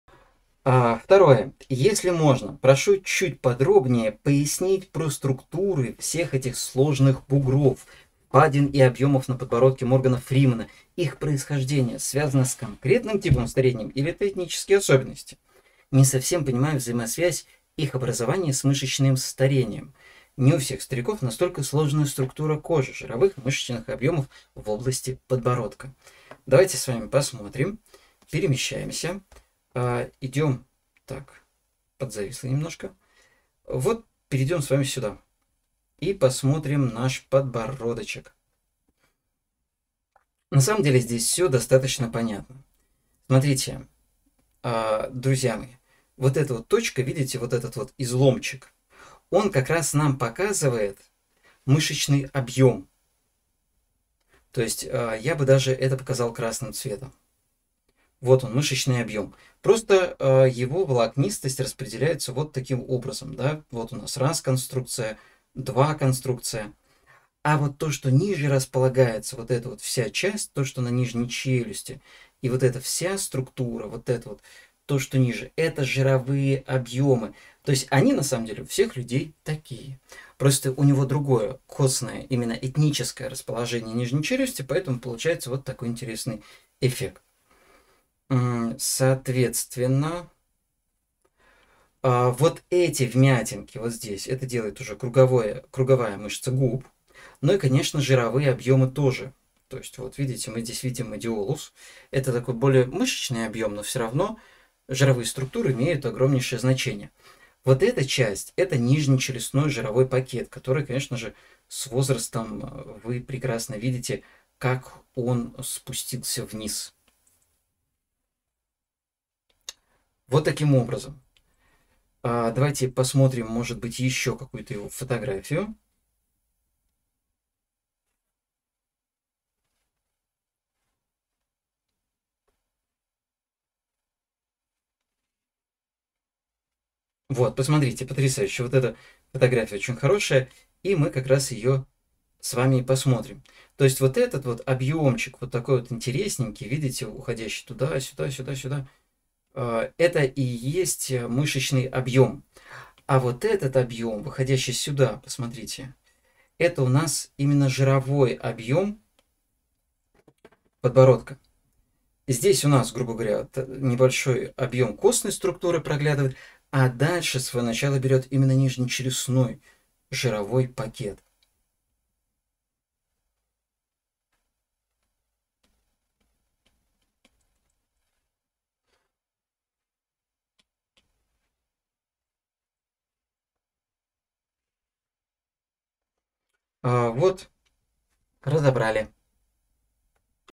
Структуры сложных бугров, впадин и объемов на подбородке.
Пояснение про структуры сложных бугров, впадин и объемов на подбородке. Их происхождение: тип старения или этнические особенности

Расписание наших курсов
Санкт-Петербург, Москва, онлайн: